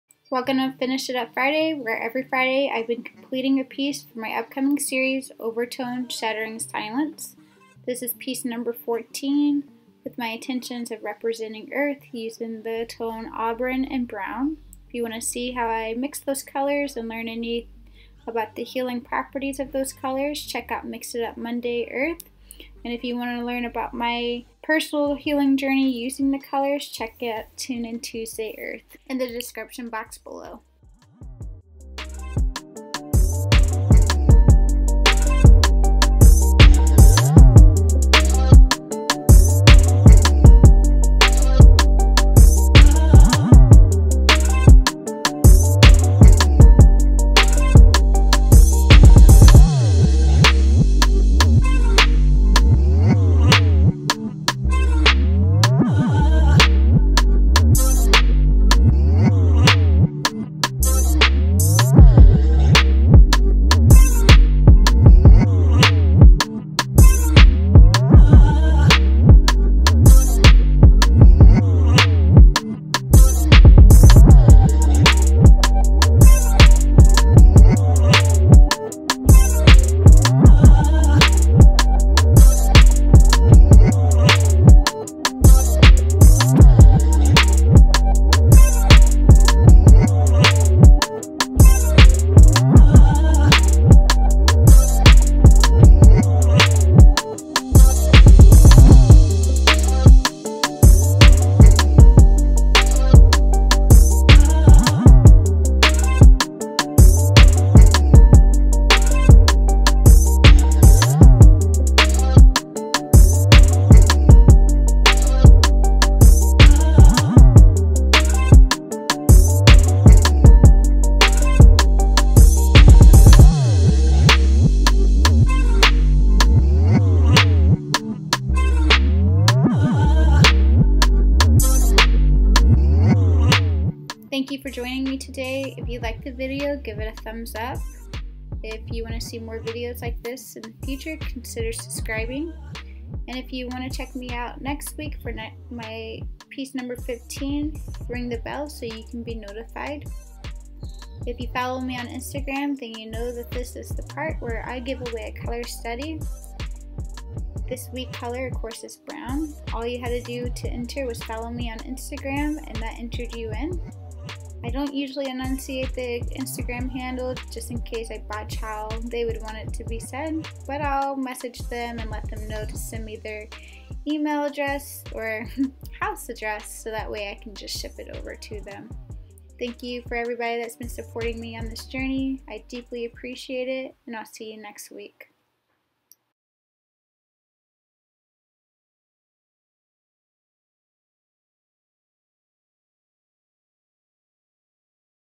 Finish It Up Friday: Brown & Auburn / Watercolor / Painting / Time Lapse
"Finish It Friday" Where every Friday I finish a painting with a new color that I mixed on Monday and started on Tuesday. Together we go in depth to learn together the ways color heals and connects to our senses. As well as me sharing with you internal process I have while painting with the color.

BROWN & AUBURN

 Overtone "Shattering Silence" 17 Works of Art on 3ft Round Glass I believe silence can change any course with a complex compromise between subconscious and consciousness. With this belief, silence creates perception and how we physically interact. Silence that is never heard like an overtone without the laughter, movement, fear and anything between like harmonics. The paintings I created are my reflection of this belief. The colors are silence that will be carried by perception from the subconscious and consciousness creating harmonics in any course that they be placed.